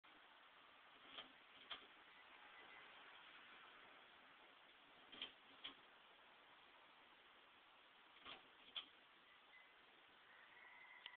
Miller sign with (2) F14T12 preheat lamps
Miller sign with 2 F14T12 preheat lamps on shop wall.